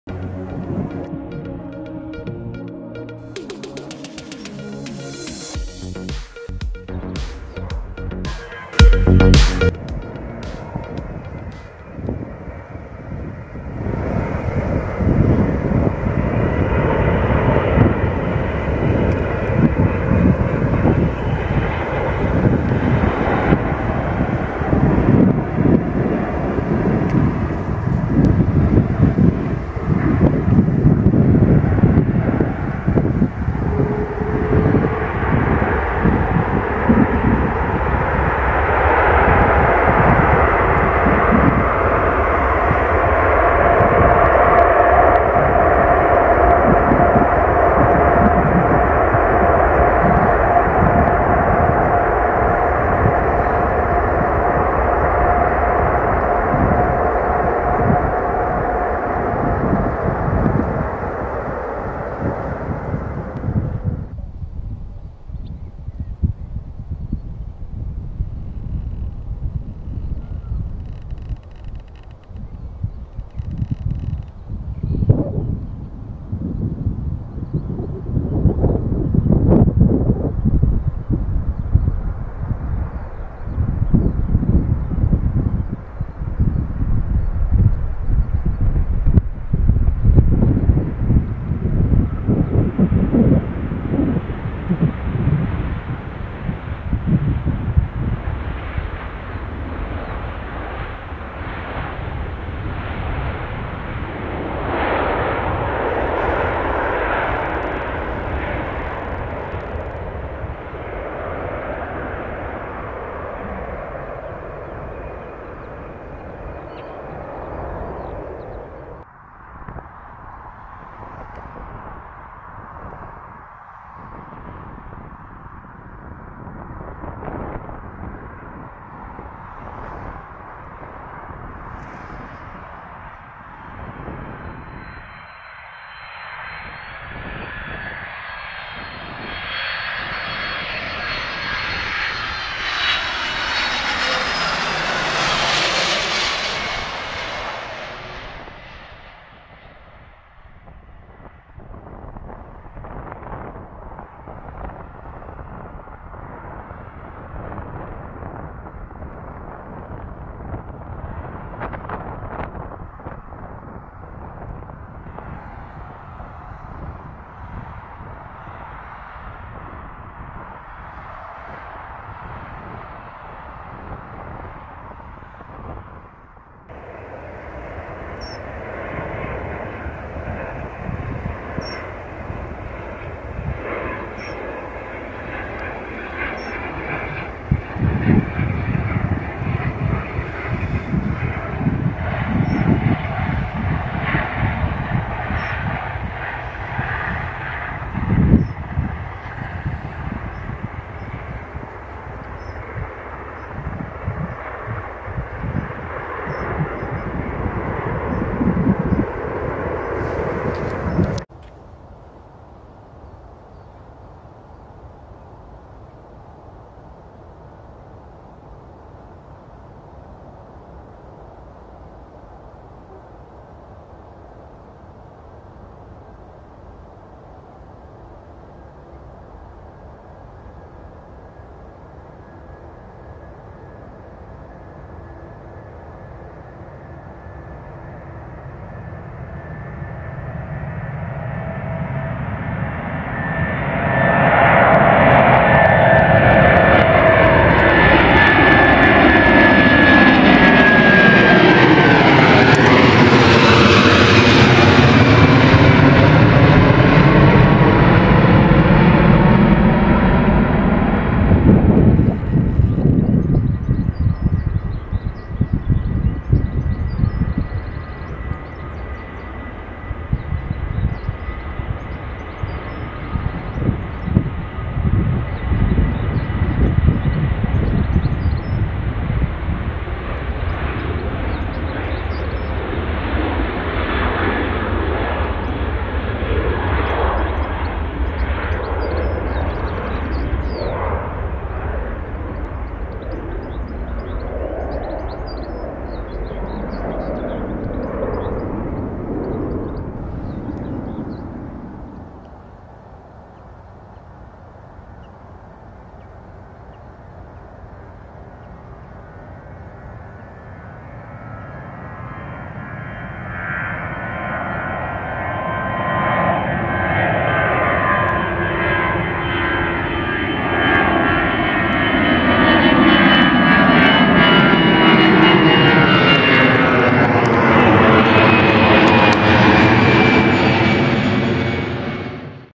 Landing and close up takeoff aircrafts in England | Manchester and East Midlands Airports
Planespotting in England Manchester and East Midlands Airports

helped me to create this video. Many thanks for the responsiveness and understanding of people in the military situation in Ukraine.

00:00 Intro
00:10 Airbus A380-861 Emirates - landing
01:04 787-9 Dreamliner Saudia Airline  - landing
01:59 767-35E(ER)(BCF) Cargojet Airways - crosswind landing
02:57 ATR 72-500 Blue Islands - Smooth landing
03:31 Airbus A330-343 Virgin Atlantic - takeoff Close UP
04:21 Boeing 737-8AS Ryanair - takeoff
05:00 Airbus A320-214 EasyJet - takeoff Close UP

My camera: Panasonic HC V770EE-K
My micro: Rode VideoMic Go